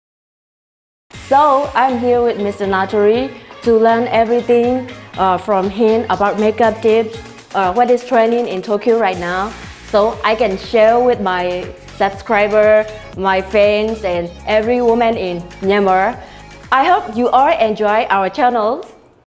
「Tokyo Beauty Channel」Myanmar Top beauty Blogger Phyo Pyae Sone Htun.Message about this channel.
This channel is makeup content by Myanmar's top beauty blogger Phyo Pyae Sone Htun and Japanese makeup artist Hitomi Natori.

Content to tell Tokyo's trend makeup to Myanmar women.

ミャンマーと日本のビューティーコラボコンテンツ。ミャンマーのトップビューティーブロガー Phyo Pyae Sone Htunと日本のメイクアップアーティスト名取瞳でお伝えする美容動画コンテンツです